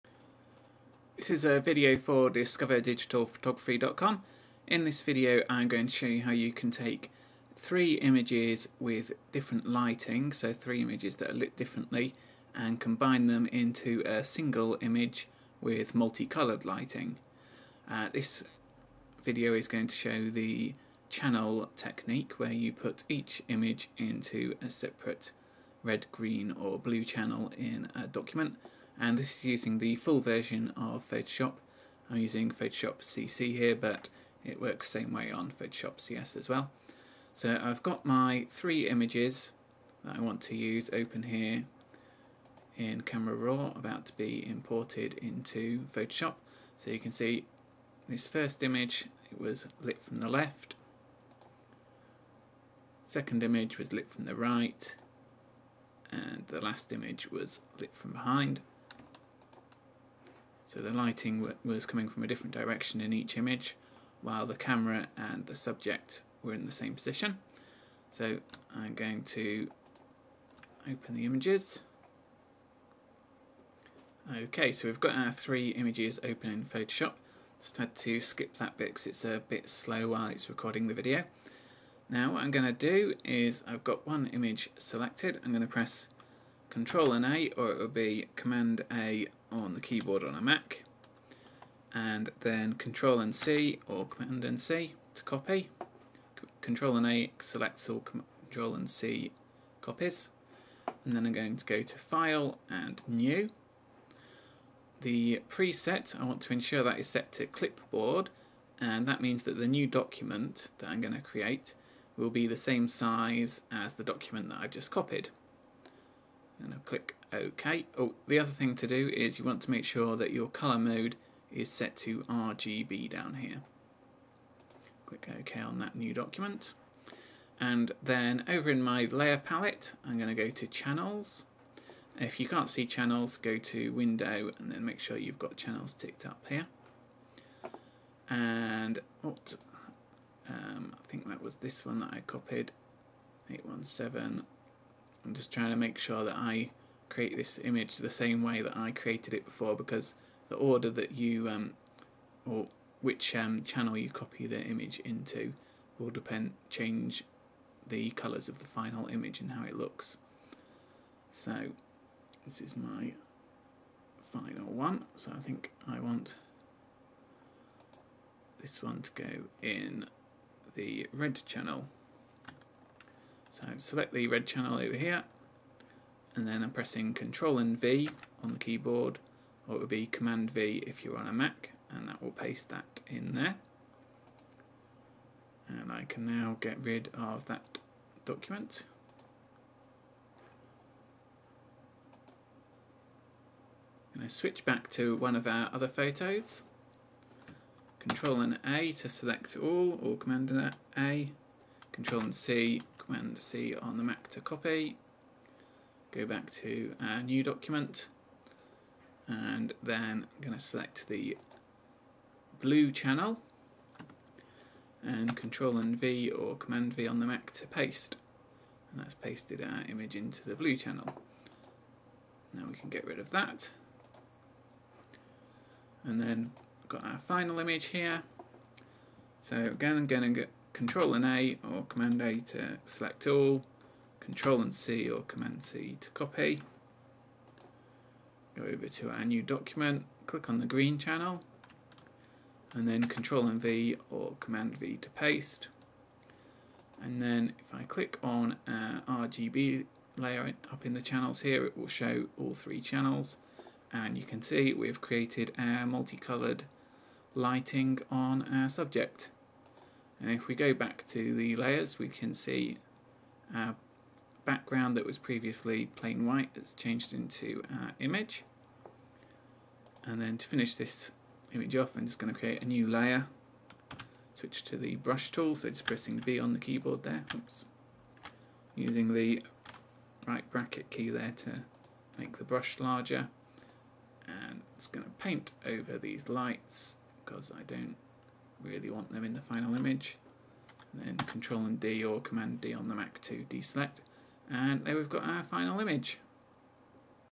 Creating multi colored lighting using RGB channels in Photoshop
How to take three differently lit images of a static subject, then combine them into a single image with colorful lighting. This video shows the RGB channels technique, which can be used in the full version of Photoshop (CC / CS).

The video accompanies the article 'Create crazy colored lighting with a single uncolored light', which can be found